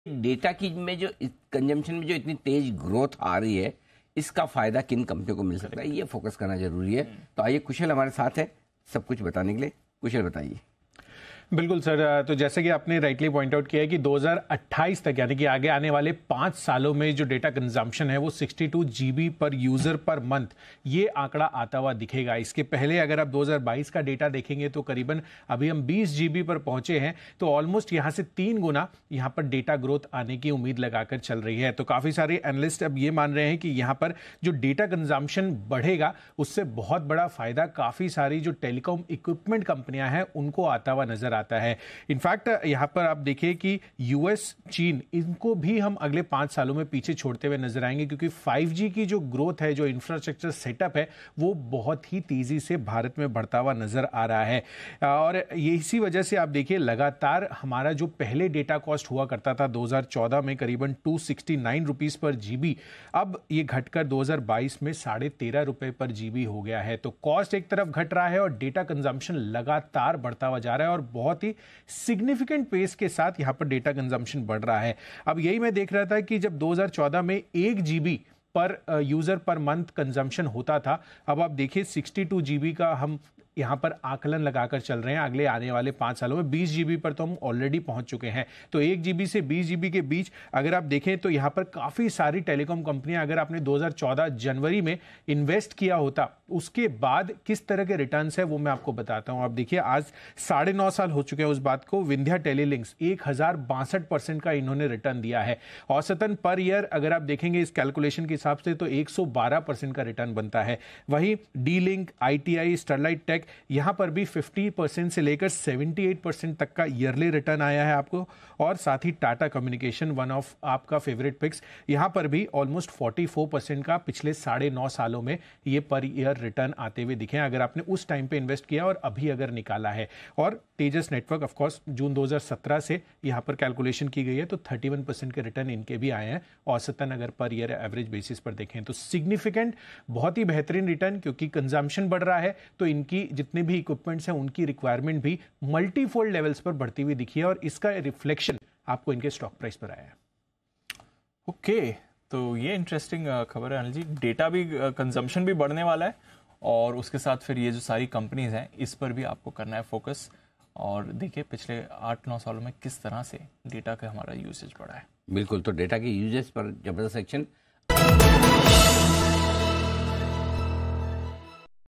India’s journey from 1GB to 62GB per month data: A look at telecom sector’s growth so far
🤳DataConsumption की रेस में सबसे आगे

प्रति महीना तेजी से बढ़ रहा Data खर्च 🛜

2028 तक 62 GB/महीना खर्च करेंगे डाटा

2028 तक 97% लोगों के पास स्मार्टफोन